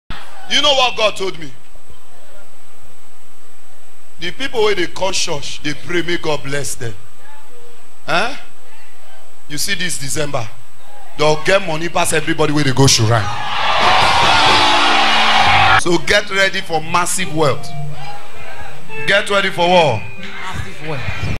Massive Wealth
Prophetic Declaration From Papa (Pst. Godson Odion) December 2020